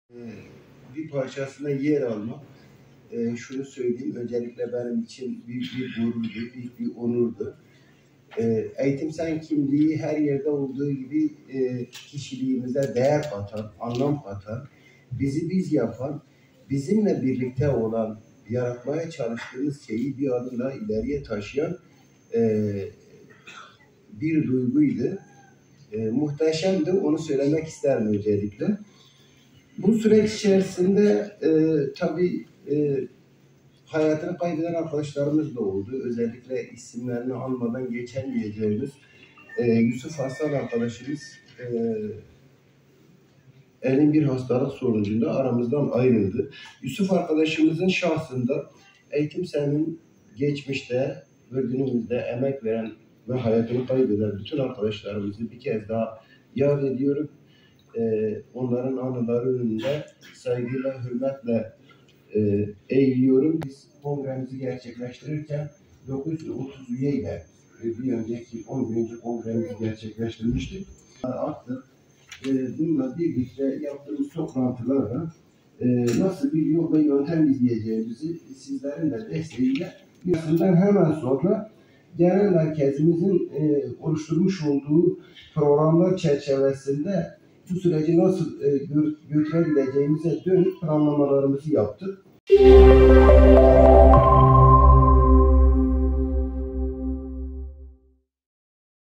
Şanlıurfa’da eğitim sendikası başkanı bırakıyor
Eğitim-Sen Şanlıurfa Şubesi 12. Olağan Genel Kurulu bugün yapılıyor. Şube Başkanı Mahmut Binici, görevini bıraktı.